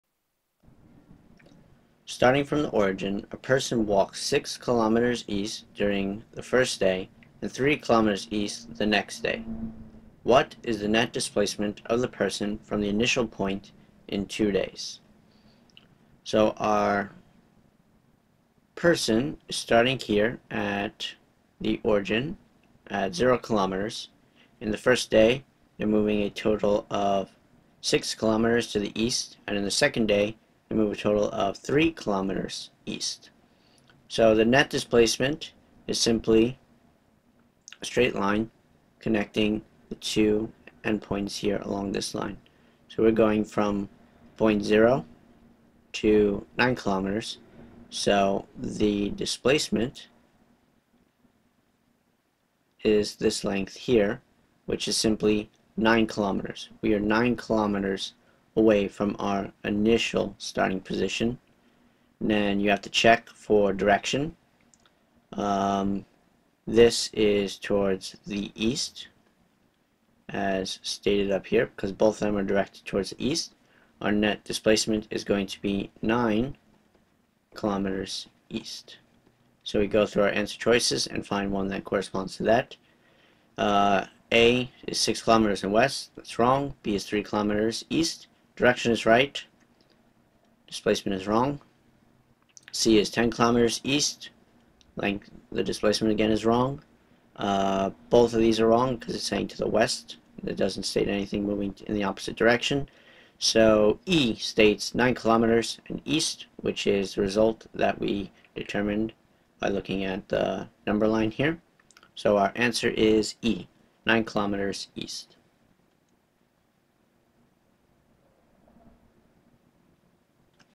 AP Physics B Kinematics Presentation #43
Starting from the origin, a person walks 6 km east during first day, and 3 km east the next day. What is the net displacement of the person from the initial point in two days?

Slide text:  43 Starting from the origin, a person walks 6 km east during first day, and 3 km east the next day. What is the net displacement of the person from the initial point in two days? A 6 km, west B 3 km, east C 10 km, east D 5 km, west E 9 km, east